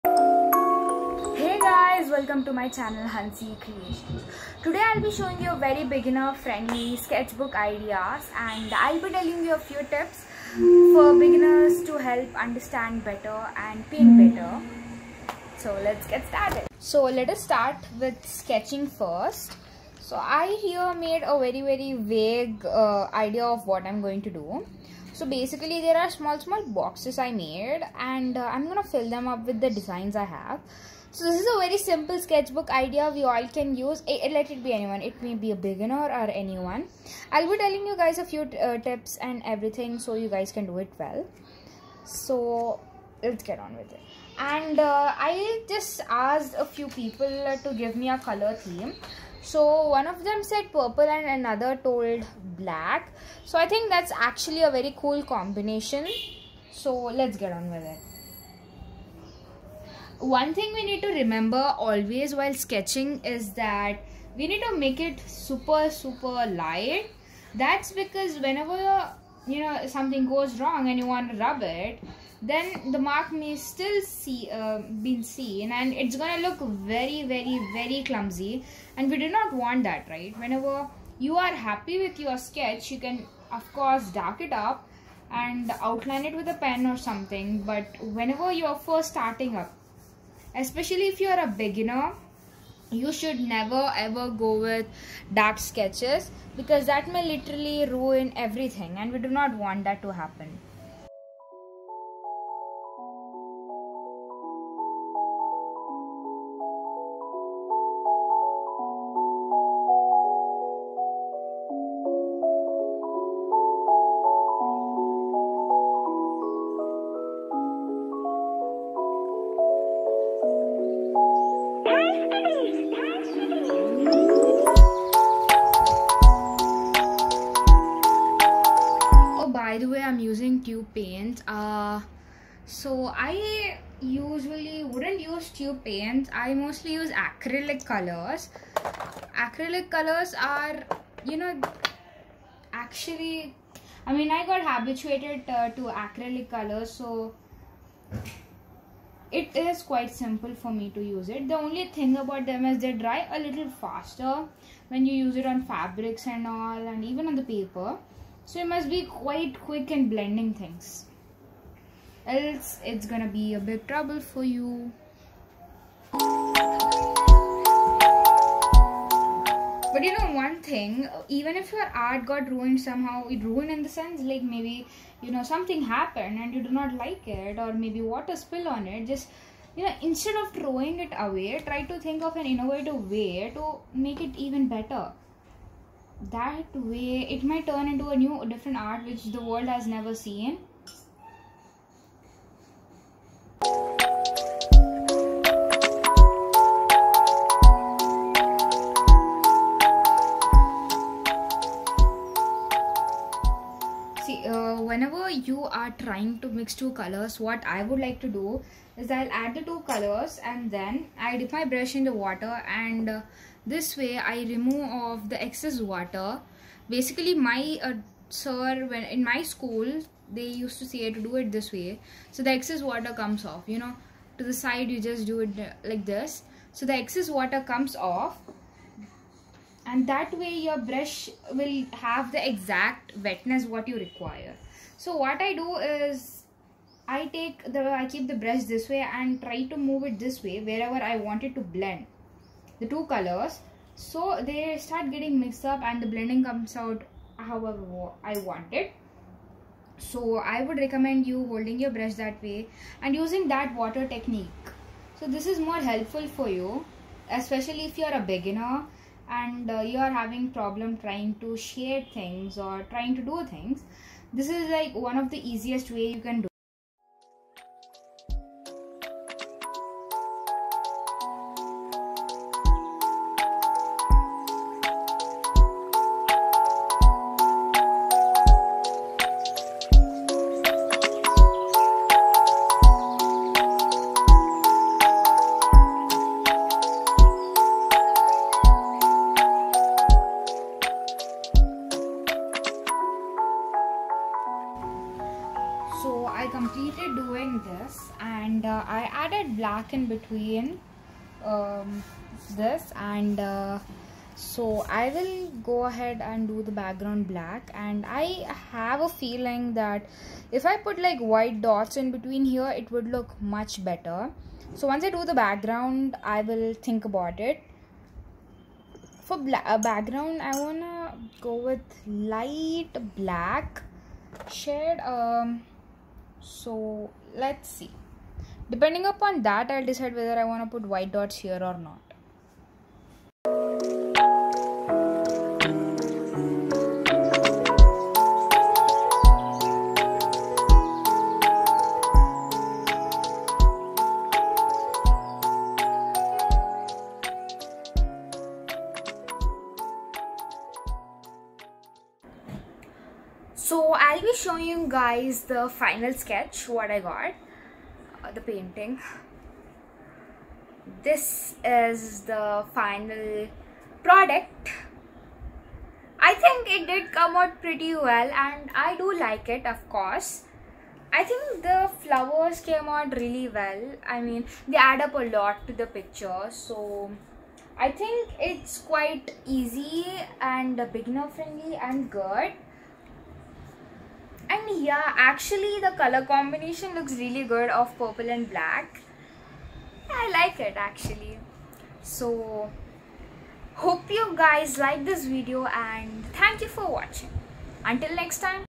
Paint With Me
Hey all! Hope you are doing well. This is a very simple sketchbook idea .This is my pinterest account if u guys would like to see new ideas ,u can check it out.